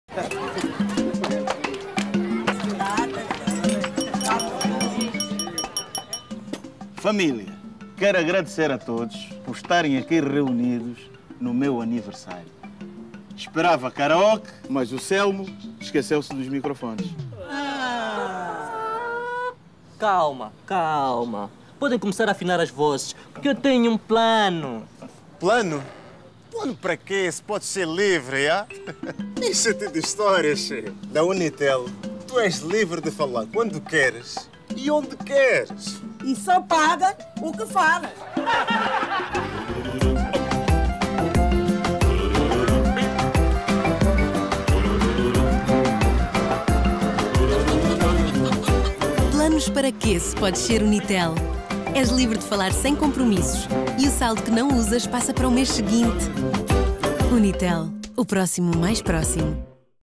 Unitel Rede Livre (Aniversário)
O Telmo e o Celmo estão de volta...

... porque a vida é feita de escolhas e quem escolhe a Unitel sente a diferença, por isso com a Unitel, o saldo que não usas, passa para o mês seguinte.